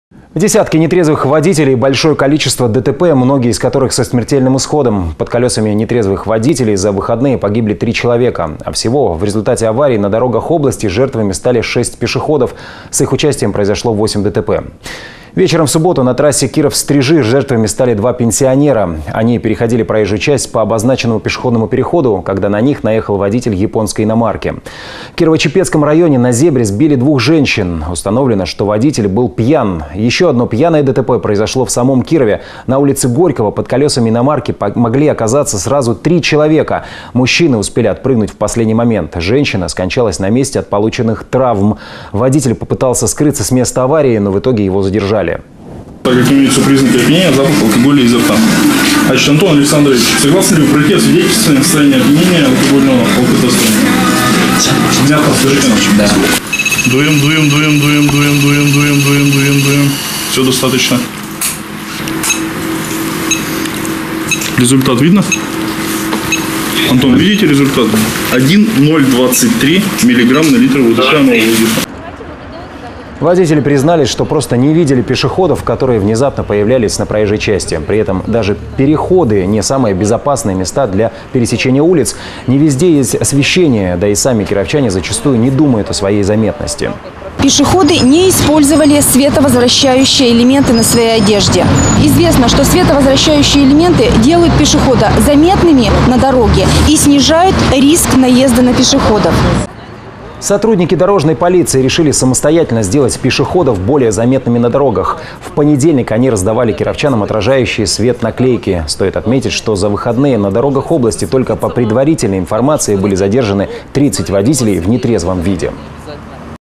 Аварийные выходные. 09.10.2017. ИК "Город"
30 пьяных водителей и большое количество ДТП, многие из которых — со смертельным исходом. Под колесами нетрезвых водителей за выходные погибли 3 пешехода